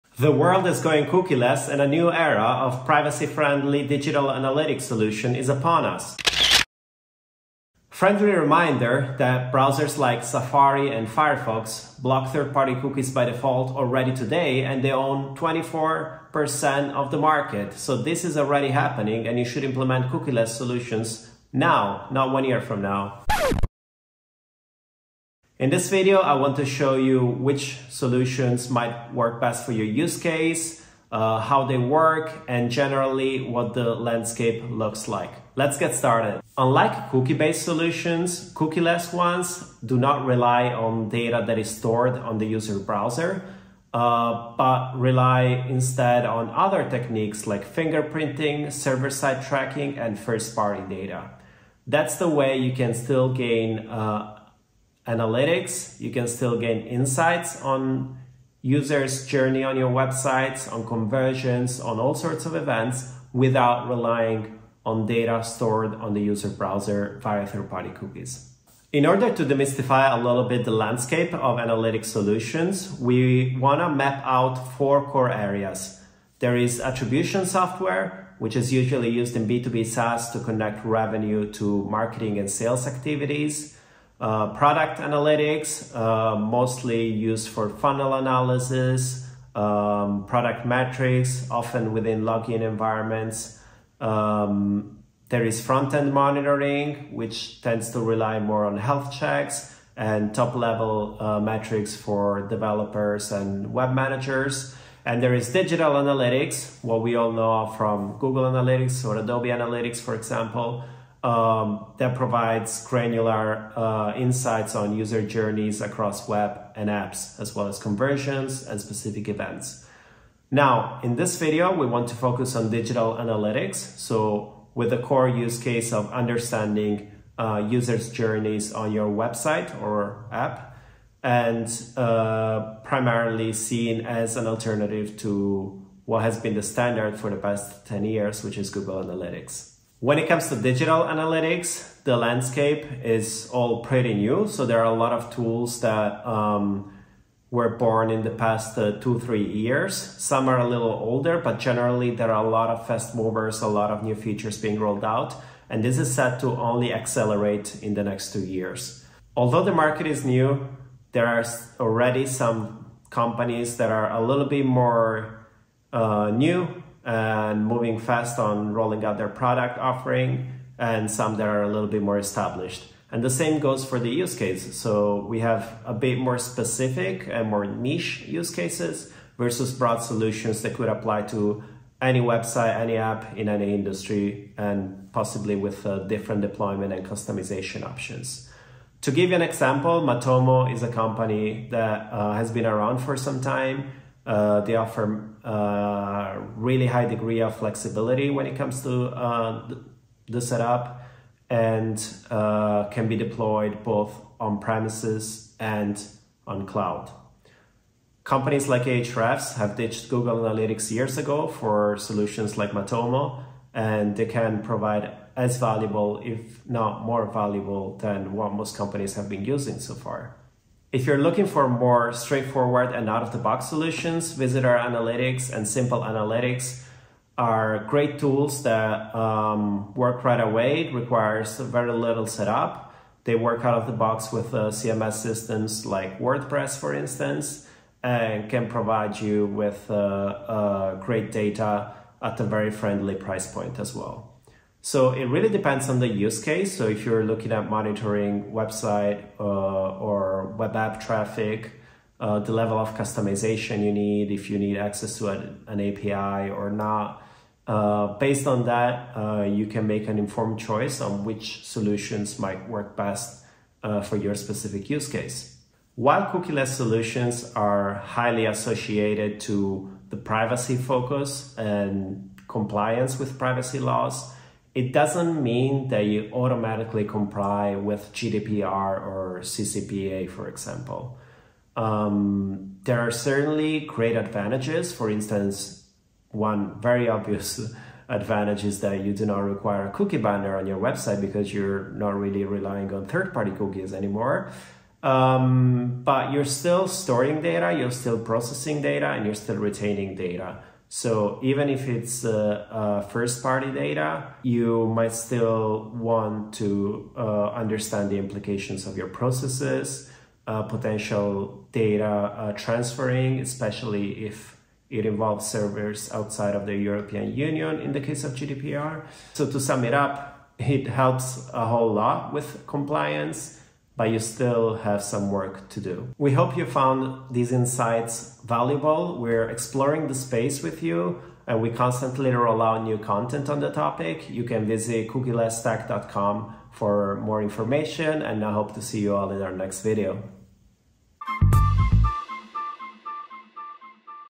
How to Choose the Right Cookieless Analytics Solution
The world has gone cookieless, and so will you.

In this video we cover:

- The shift from traditional cookie-based tracking to privacy-friendly alternatives.
- An overview of cookieless tracking techniques such as digital fingerprinting, server-side tracking, and first-party data collection.

- A  look at the top cookieless tracking solutions in the market, like Visitor Analytics, Matomo, and others.

- Key factors to consider when choosing a cookieless tracking solution, from features and compliance to pricing and community support.

The digital marketing landscape is evolving, and staying ahead means adapting to changes. Our video provides you with the knowledge and tools to navigate this shift with confidence.

Want to learn more? for in-depth guides, insights, and the latest trends in the world of cookieless technology.